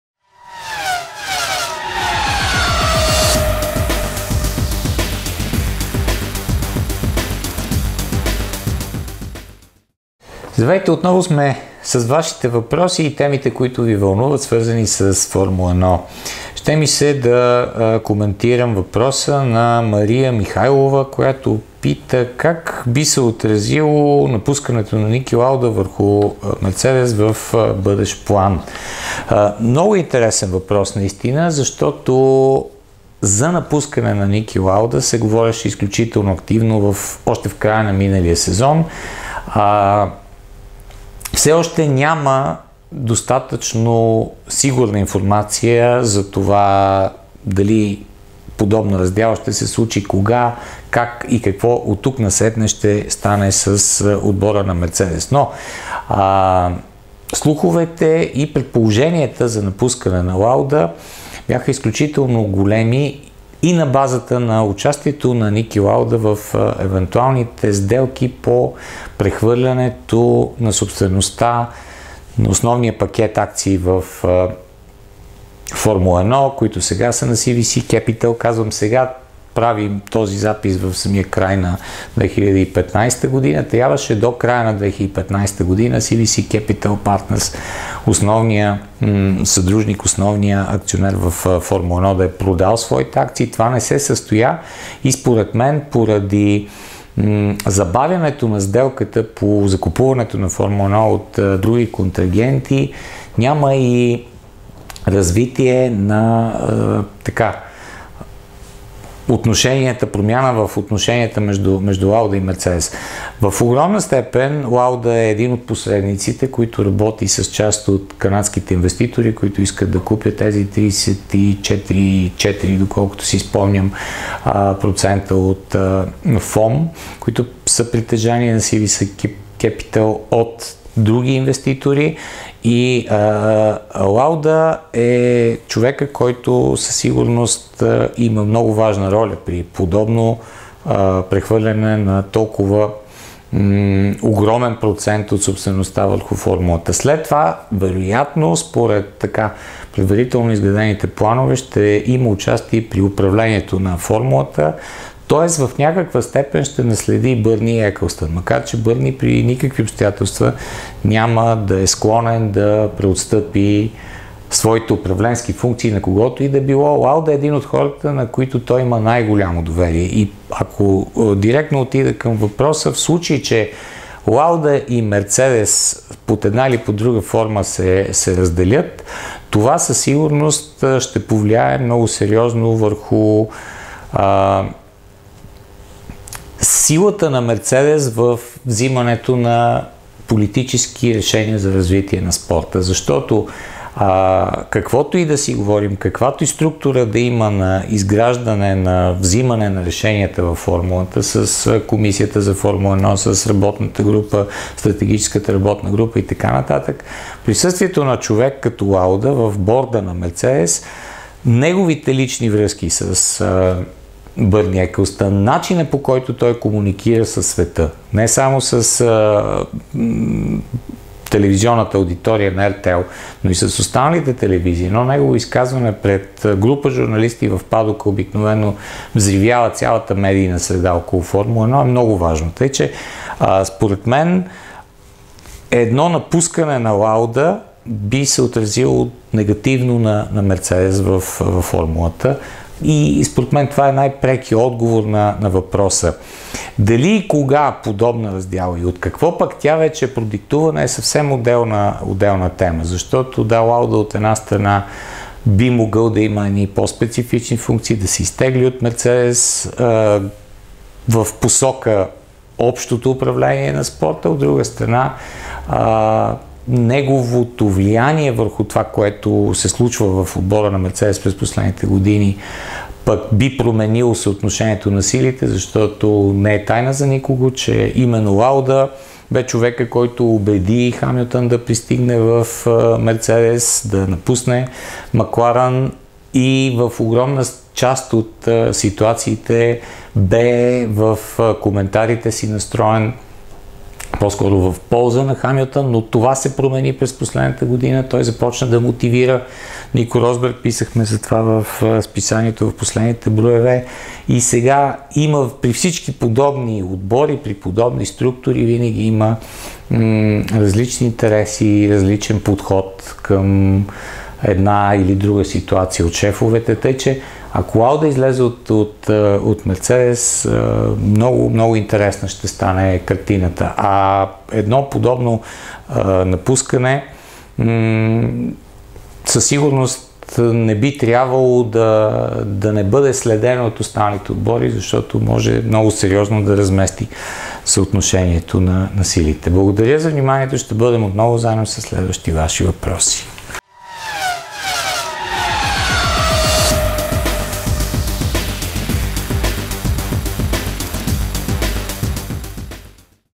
Как би се отразило напускането на Ники Лауда на Мерцедес
Темата на епизода е свързано с въпрос на Мария Михайлова:

Как ще се отрази евентуалното напускане на Мерцедес от страна на Ники Лауда?